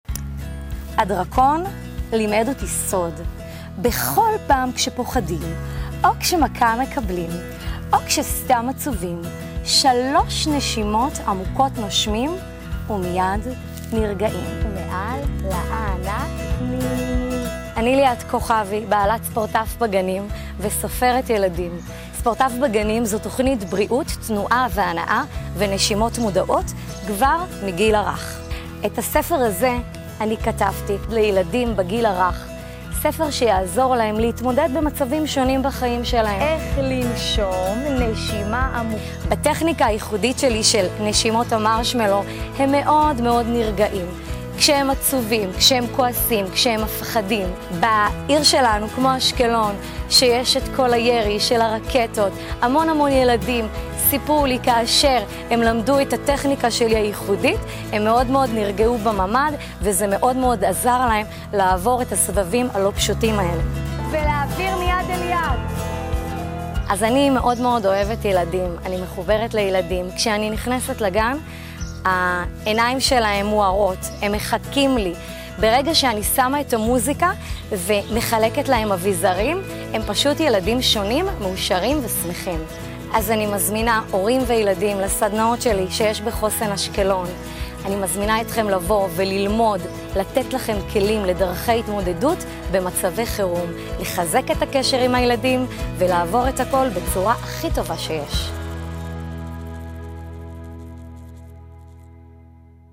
הכר את העסק - ספורטף
את הדרקון שמלמד את הילדים איך לנשום כבר הכרתם? 🐉
לליאת כוכבי, בעלת ספורטף בגנים יש שיטת נשימות מיוחדת לילדים שמסייעת להם בהתמודדויות שונות 👼🏼
תדברו עם ליאת, היא תעניק לכם כלים להתמודדות עם הילדים במצבי חירום ובצורה כיפית במיוחד 😍